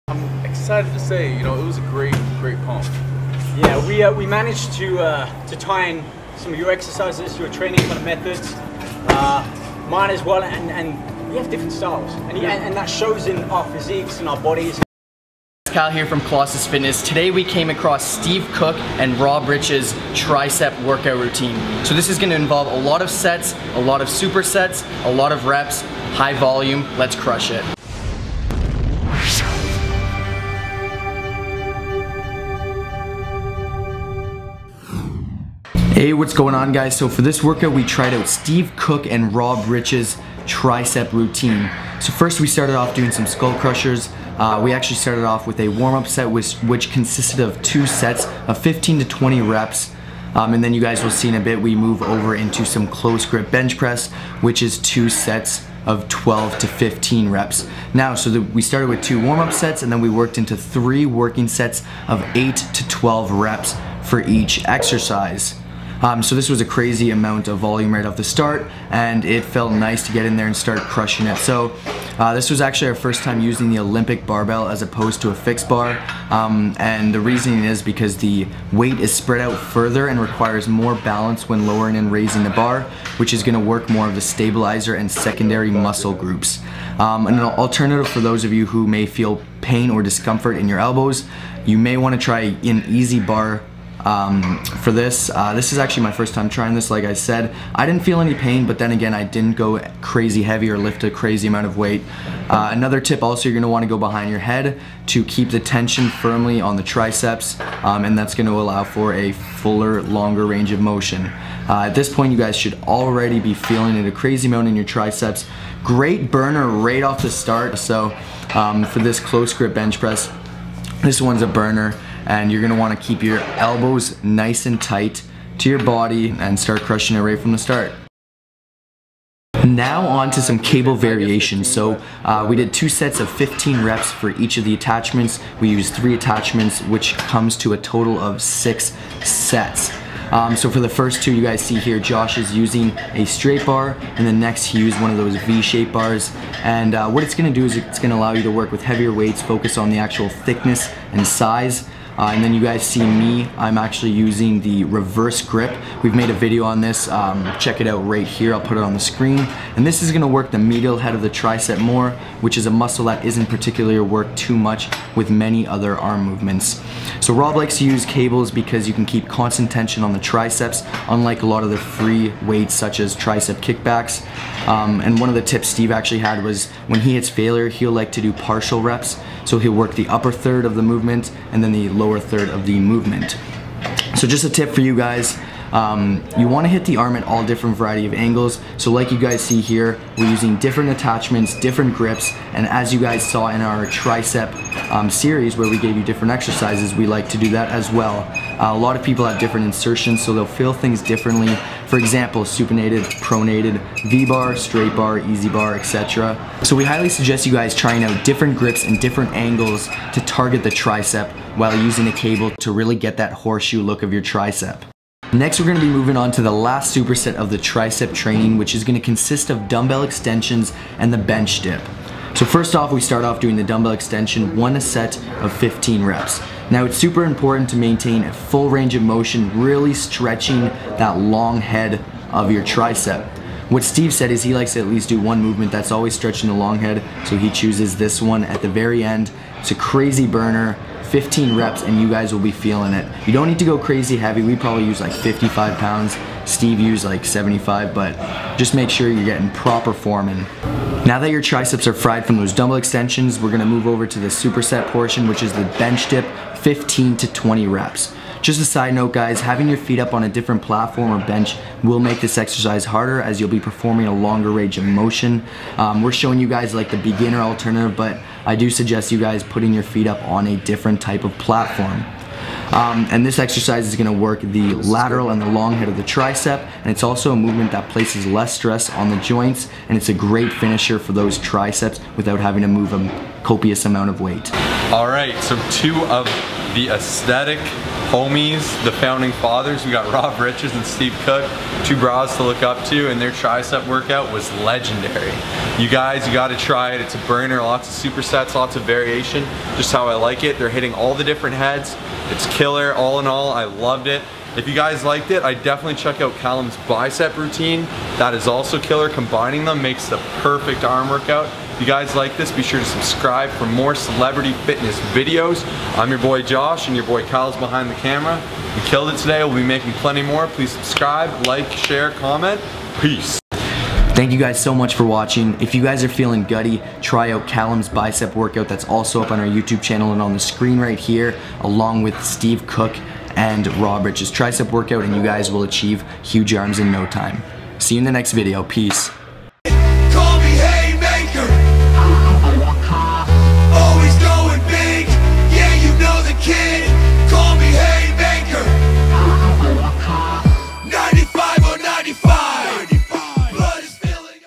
Steve Cook and Rob Riches Massive Tricep Workout Routine + Training Tips
Welcome to episode 2 of our Celebrity Fitness Workout series! For this workout we went through one of Steve Cook's and Rob Riches tricep routines that they did at the Golds Gym in Venice Beach. Right after we moved into biceps. But instead of doing their bicep routine we decided to try out Calum Van Mogers to mix things up. Check it out down below! Who do you guys want to see next??